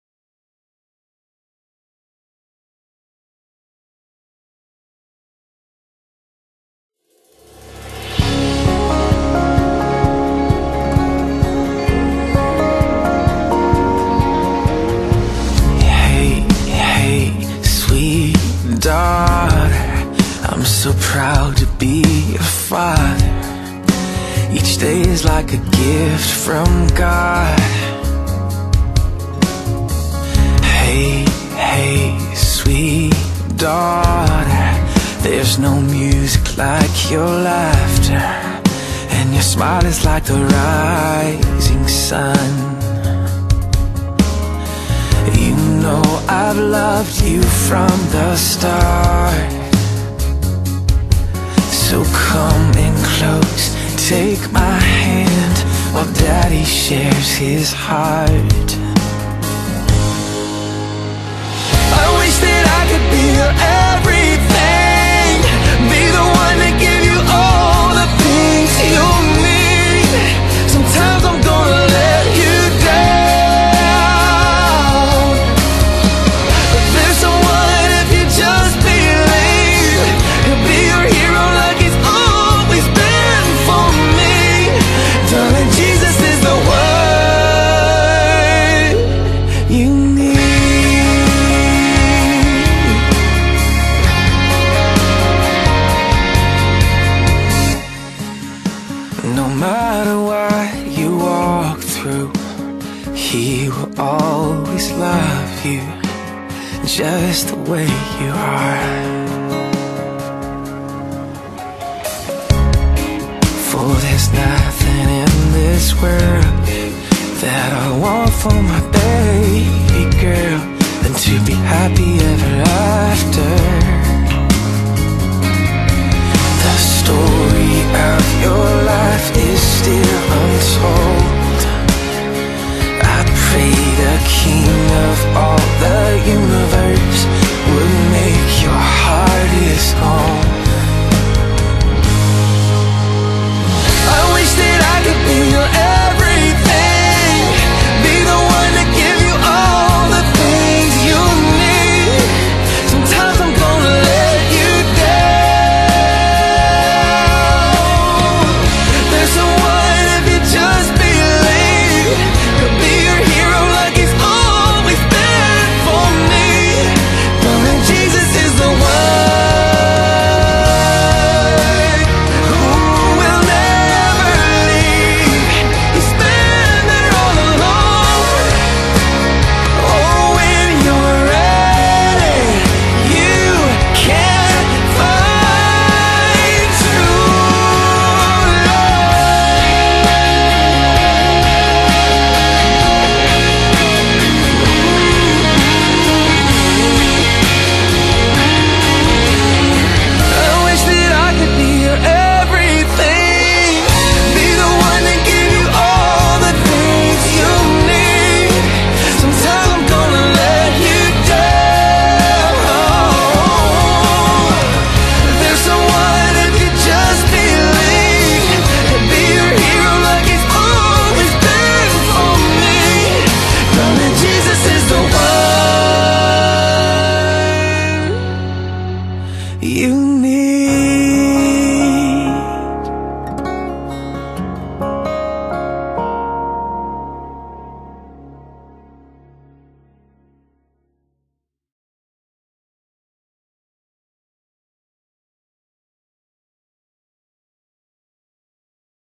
FD Dance 2013 short
280 Father and Daughters had a special date night at Delphi UMC in early March 2013.